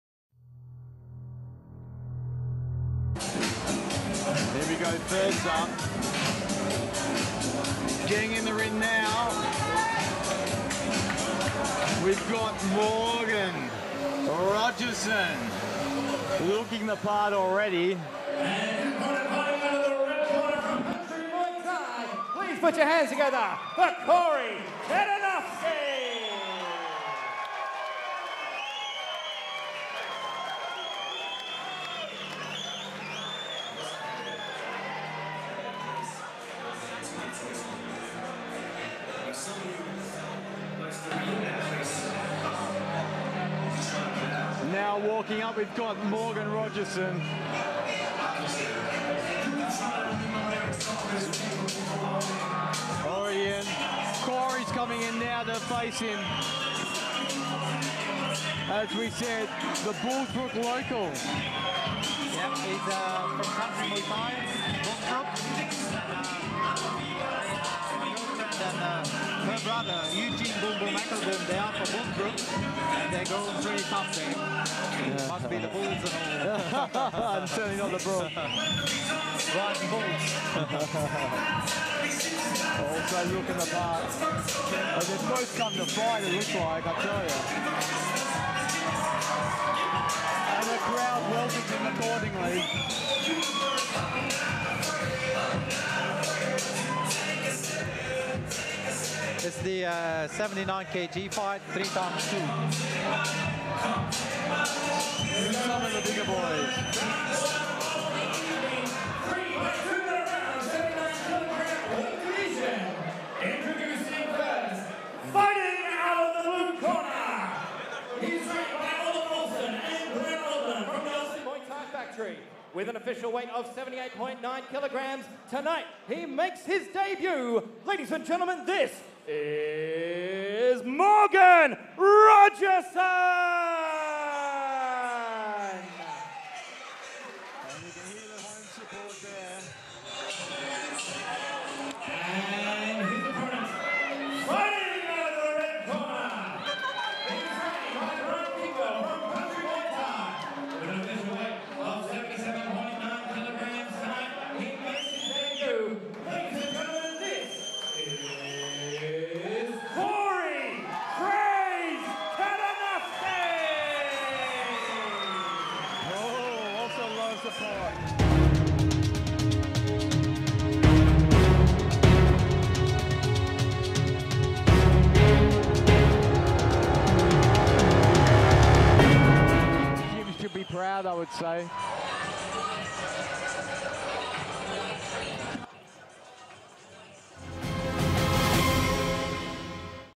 Battle Collossal XXI - Fight 1
This is a highlight from Battle Collossal 21 (May 2016)  with Kory 'Kraze' Ketternovki and Morgan Rogerson.